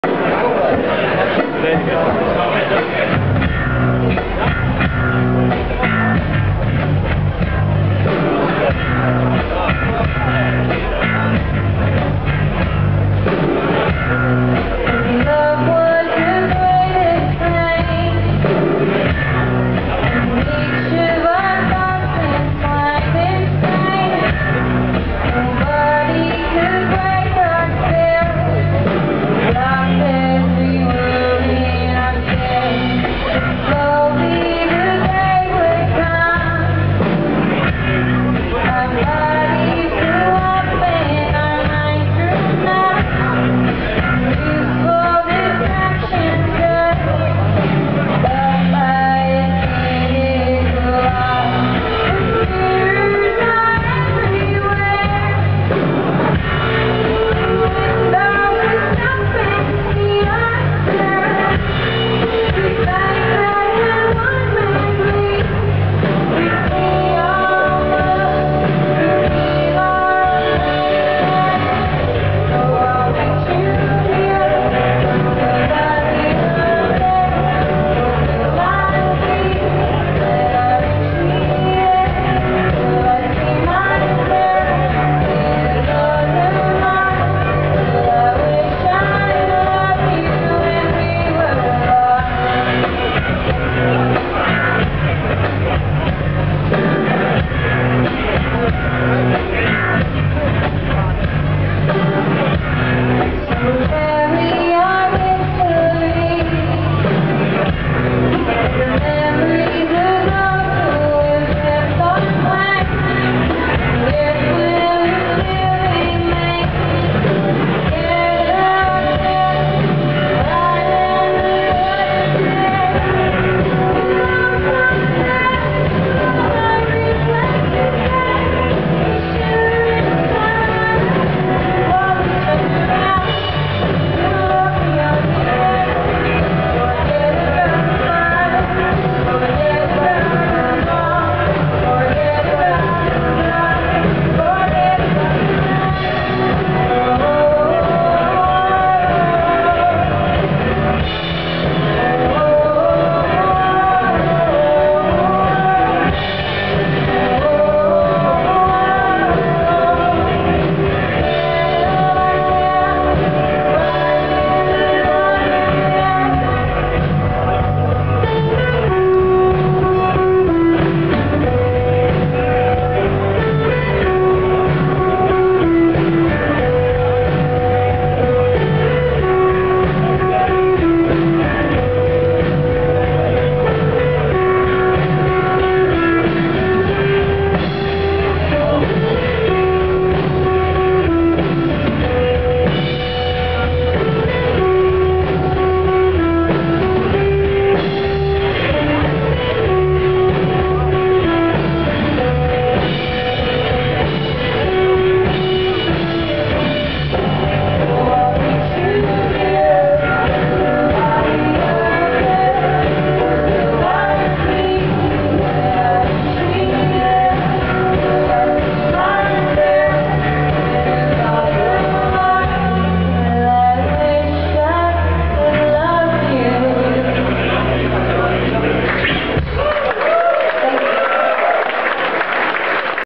Sparrow and the Workshop LIVE @ Gorilla 11/04/13
Playing support for British Sea Power.